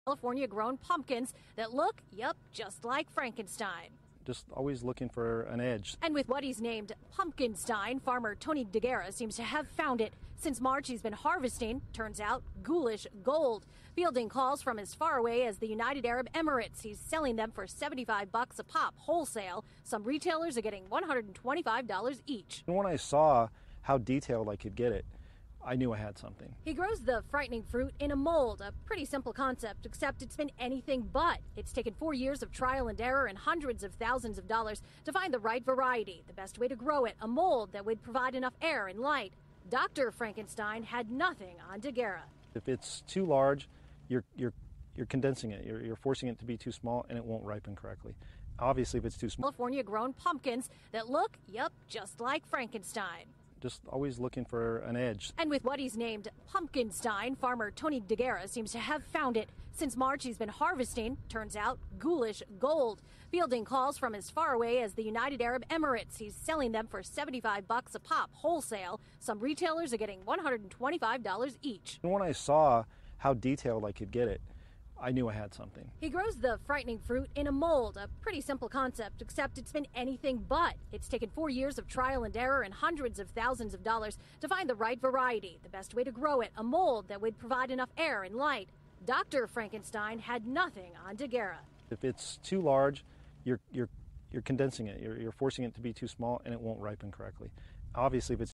Breaking News| Farmer created a monster: Pumpkinstein#2| September 23, 2014
A California farmer has created a mold that grows pumpkins that resemble spooky Halloween figures such as Frankenstein. From CNN breaking news, breaking news.

 - Pumpkinstein showcases the frightening outcome when a rotting pumpkin and Frankenstein are combined! .